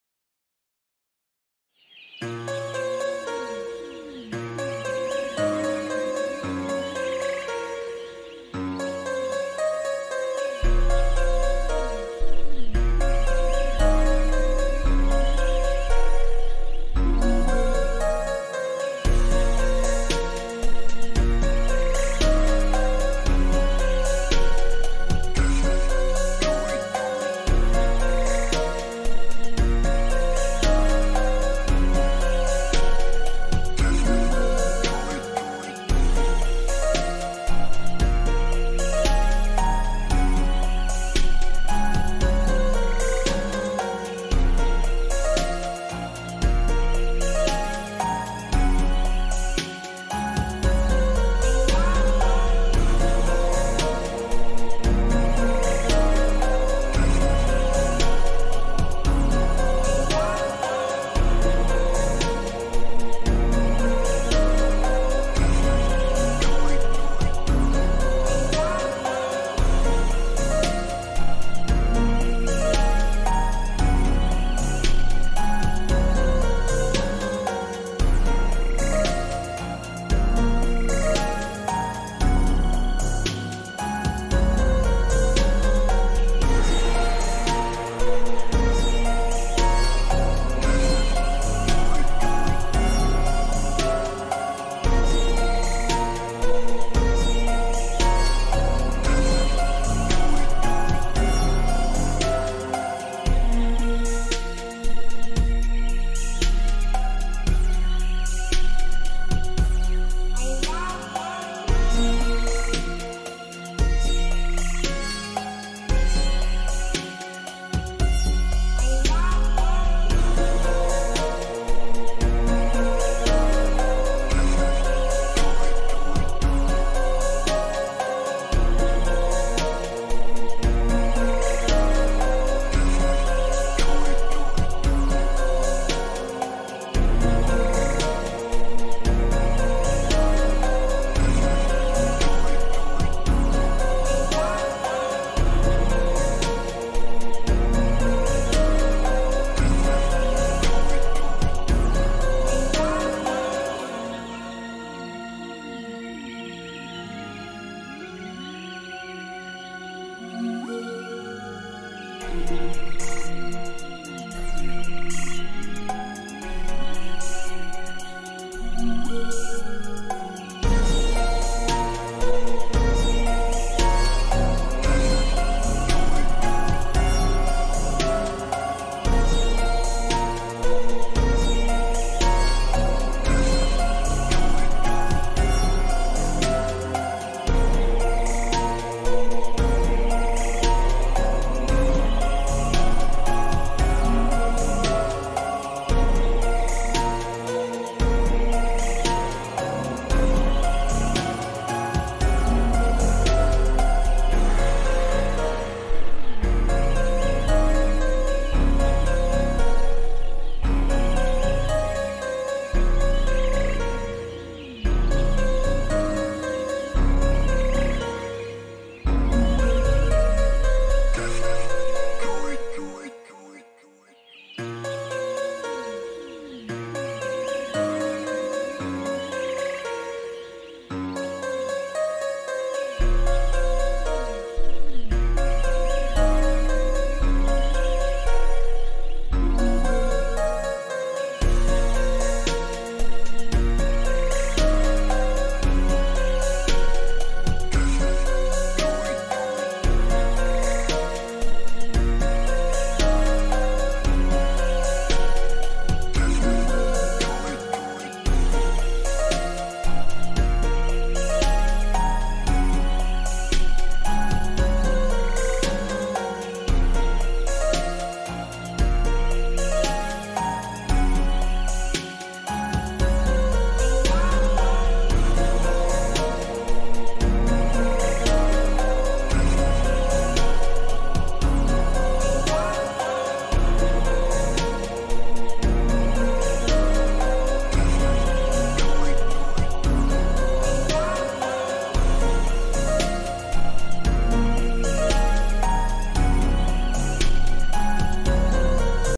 چلو گوشت گوساله(استیک)
دوستان سینی فر(که آب داخلش بود ) رو هر بار پر از آب گرم کنید تا گوشتتون نسوزه..